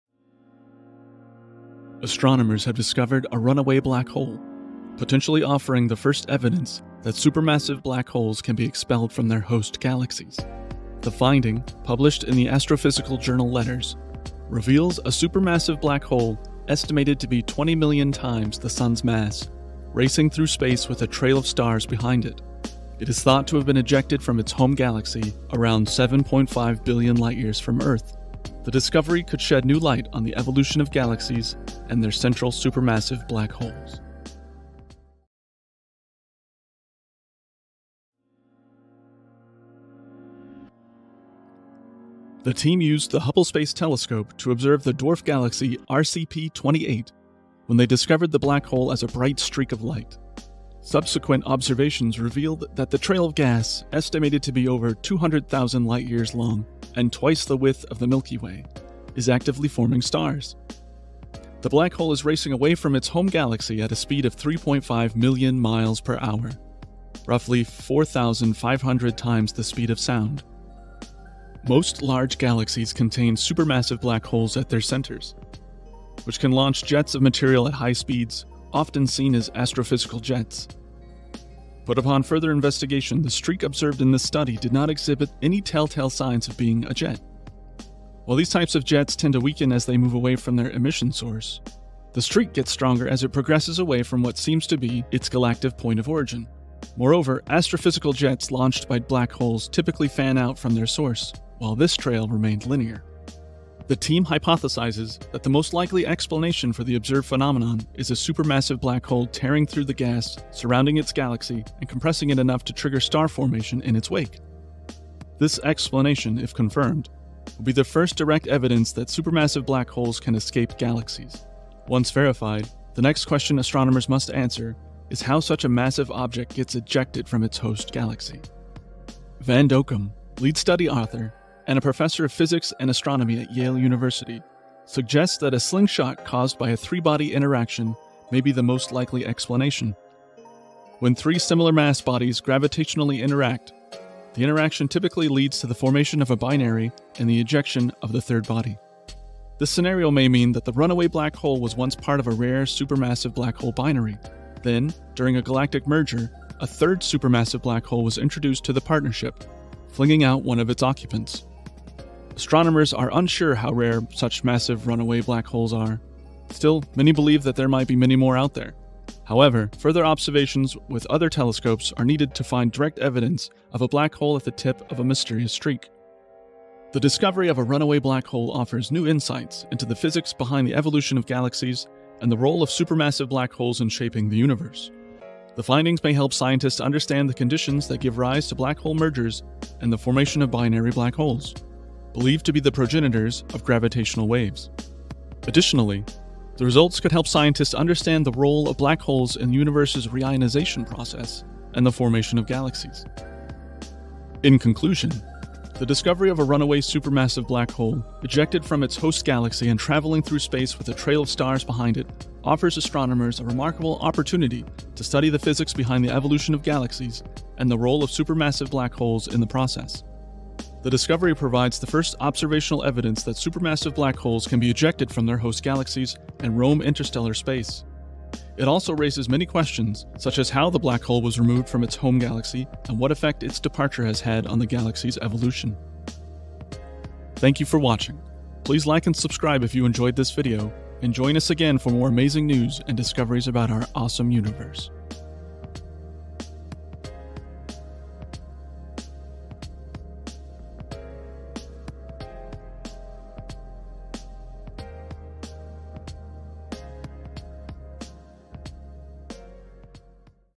NASA doesn't understand what happened to this BLACK HOLE!
NASA scientists recently discovered a blackhole that's been ejected from its own galaxy and is roaming the universe!

Image Credits: ESA/Hubble & NASA, M. Sun, X-ray NASA/CXC/IfA/D.Sanders et al; Optical NASA/STScI/NRAO/A.Evans et al